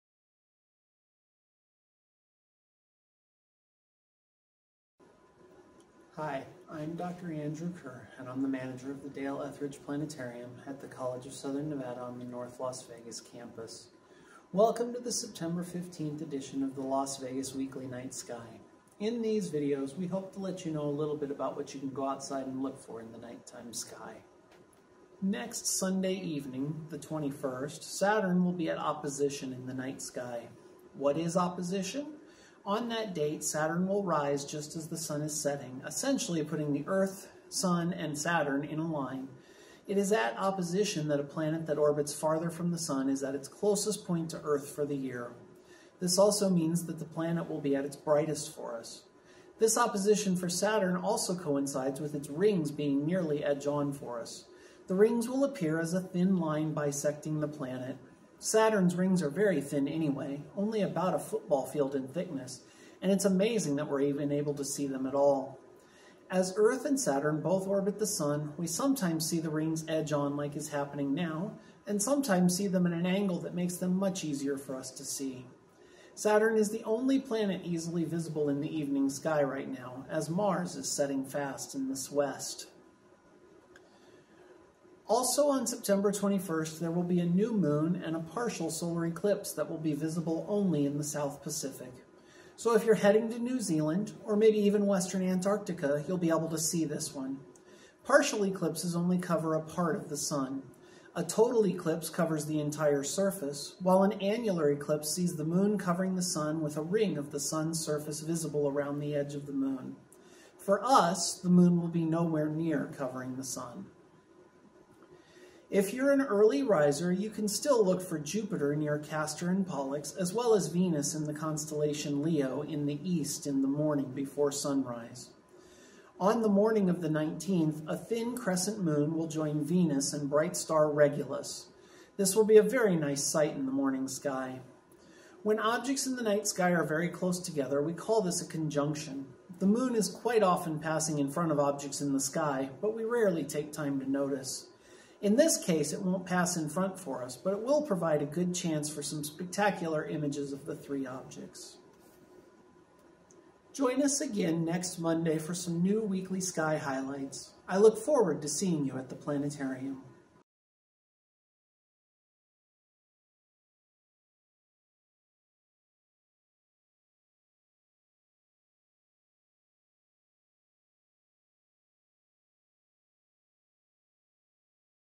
Las Vegas Night Sky Weekly: September 15th - 21st, 2025
Your Las Vegas Night Sky Weekly is here for September 15th - 21st, 2025!

Join Dale Etheridge Planetarium at CSN manager and astrophysicist, Dr. Andrew Kerr as we explore this week's night sky, with highlights including:

• Saturn at Opposition on Sunday the 21st
• The meaning of the astronomical terms Opposition & Conjunction
• An upcoming treat in the early morning sky
• A partial Solar Eclipse visible in the South Pacific & different types of solar eclipses explained
• Which planets you can view in the nighttime or morning skies

We'll see you back again next week for more night sky highlights from Las Vegas! ✨